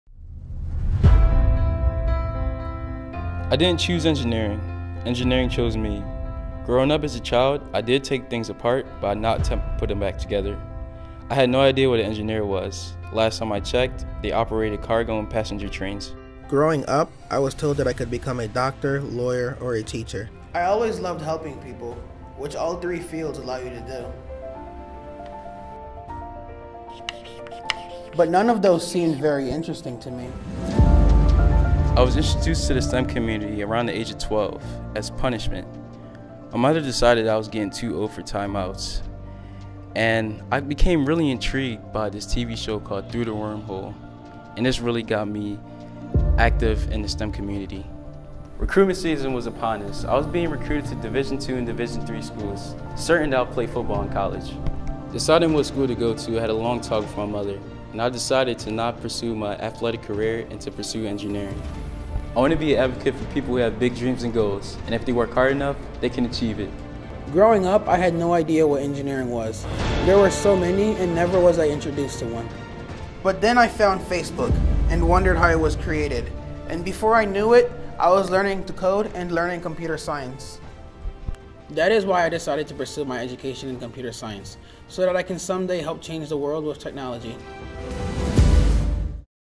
We Are Engineers
It was an honor creating this video with Jamir R. Walker. We are both engineering students at Pennsylvania State University in University Park, PA. Jamir is pursuing a degree in Aerospace Engineering and I am pursuing a degree in Computer Science. This video was recorded with a Nikon D810 DSLR camera and edited using Adobe Premiere Pro 2020.

Free Stock!!! Using Robinhood

2 Free Stocks!!! Using Webull